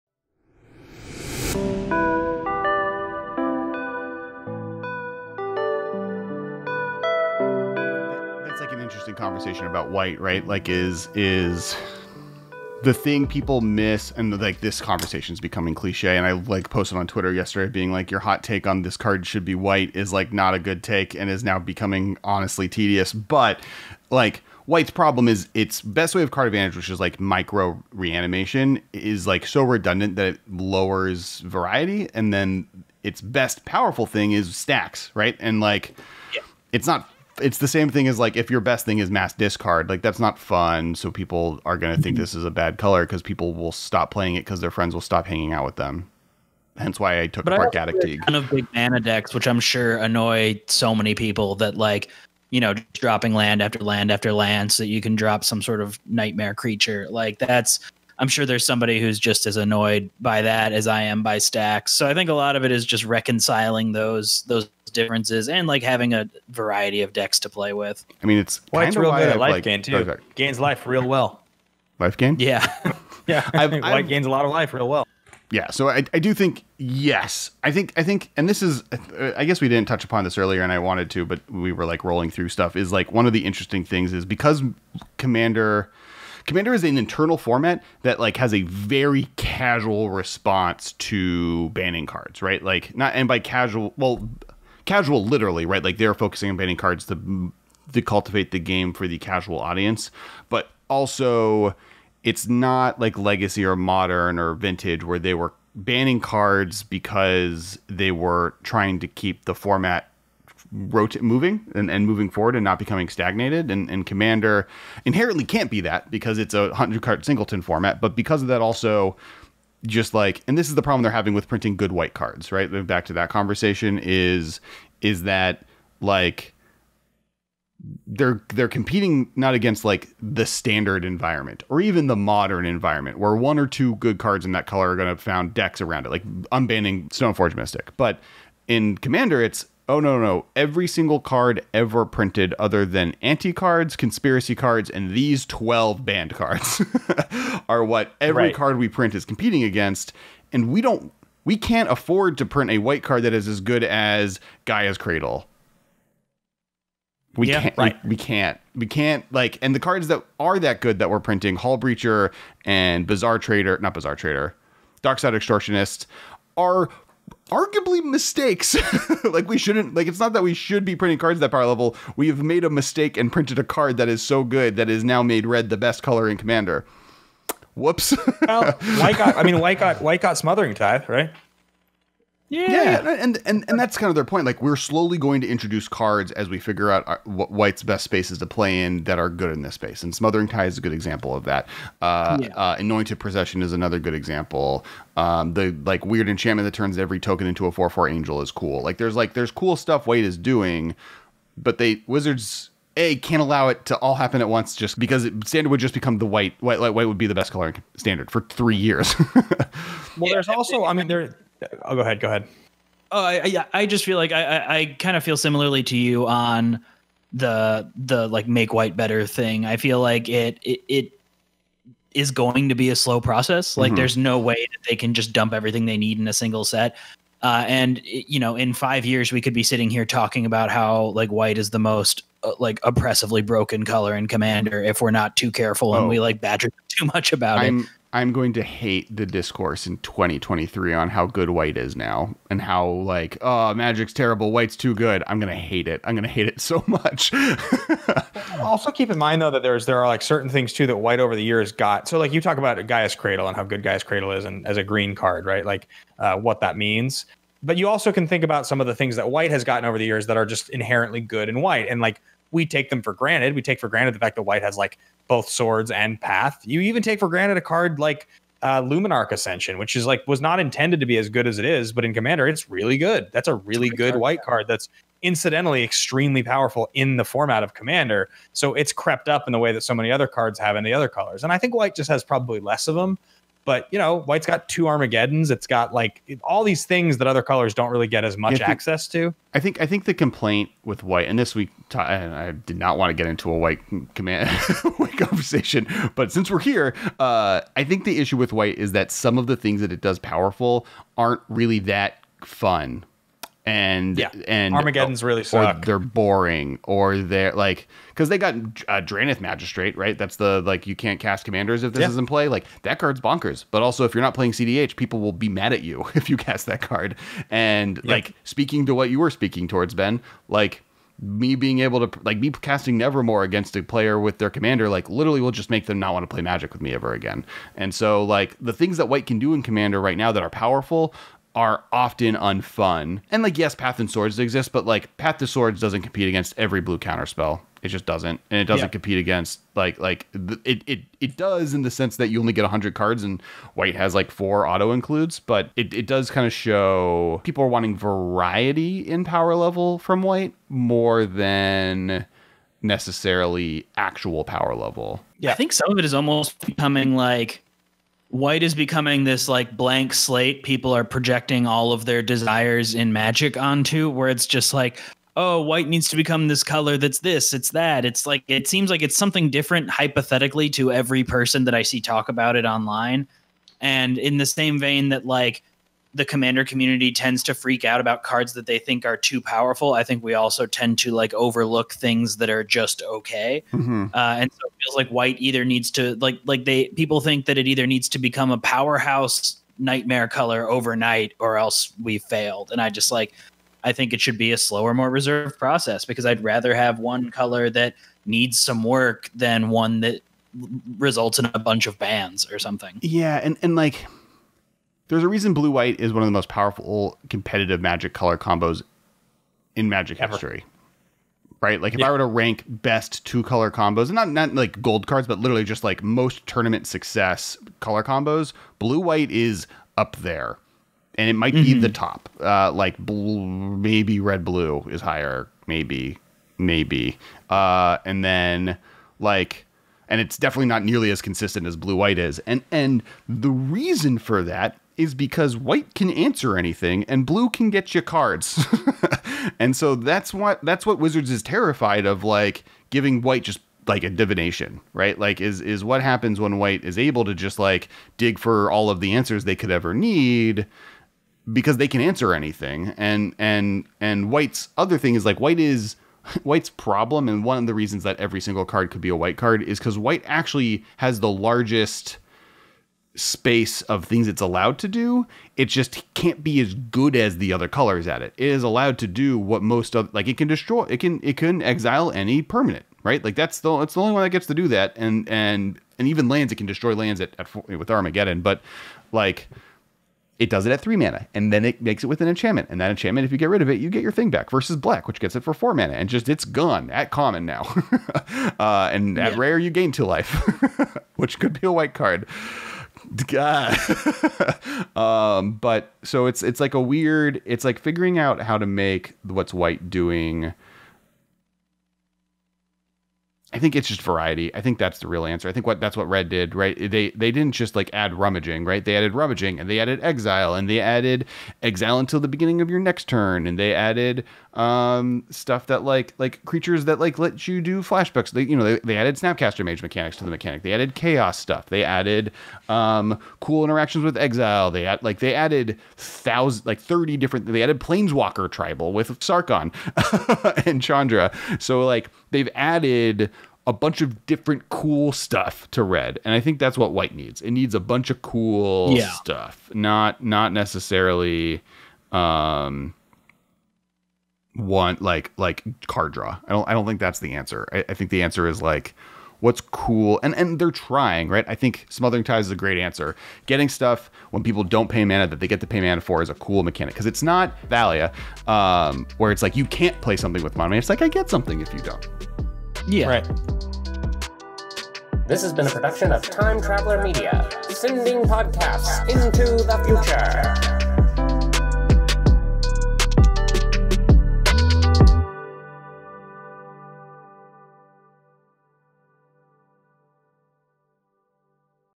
The problem with white- A Hot Take with Dan Sheehan
White has continued to slip behind power level of other colors, particularly in formats like Commander, while  Blue and especially Green rocket forward in strength. Today we explore why, and try to see a way forward for the straggling color. What do you think? Got any ideas we missed? Any ideas you'd like to see implemented to help white out?

Check out Dan and Rachel Weeks every week on their podcast, the Commander Sphere

Check out Alex Kessler's Battle Bosses miniatures combat game!